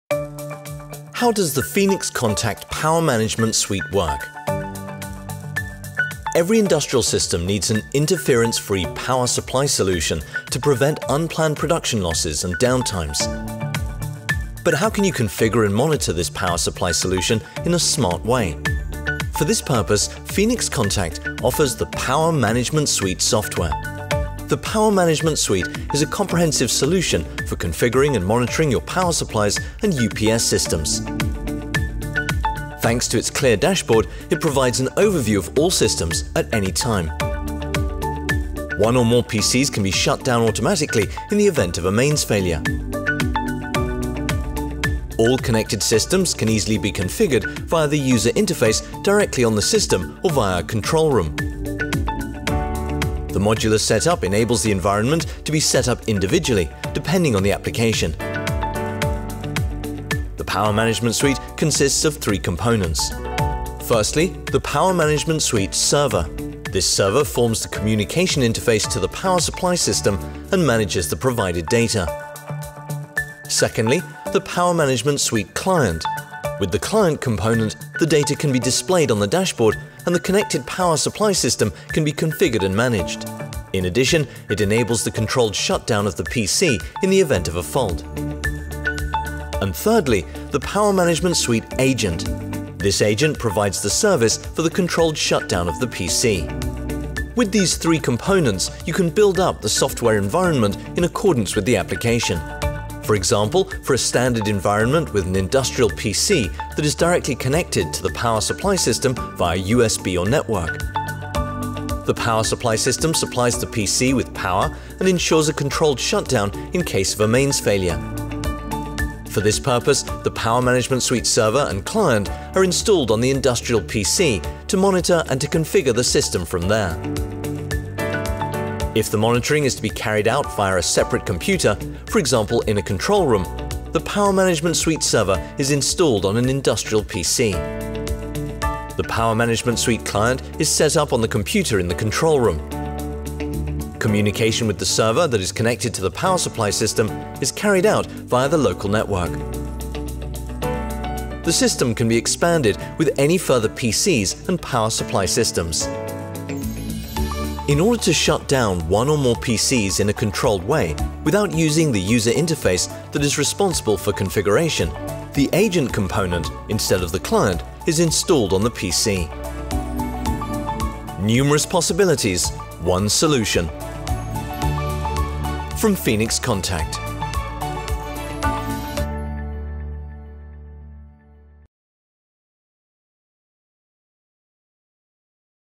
How does the POWER MANAGEMENT SUITE work?
►Every industrial plant needs a trouble-free power supply solution to avoid unplanned production downtimes and shutdowns. But how can this power supply solution be smartly configured and monitored?

►The software POWER MANAGEMENT SUITE is a holistic solution for configuring and monitoring your power supply and UPS systems.

►Learn more about the benefits and functions Power Management Software in this explanatory film.